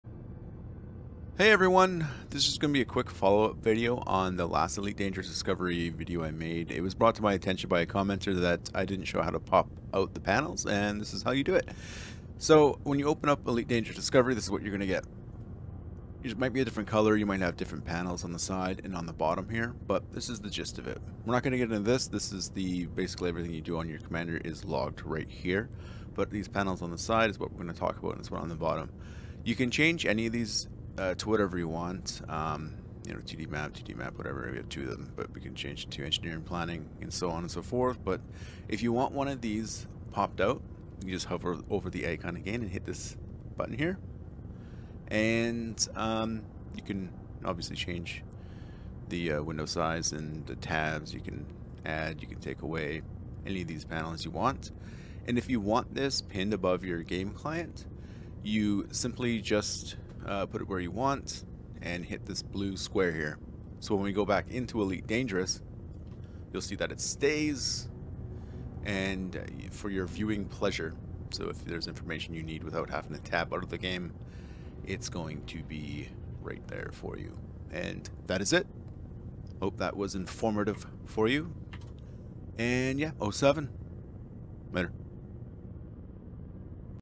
EDDiscovery Panel Pop-out
Quick follow up to this video showing how to pop out panels for your own personal need.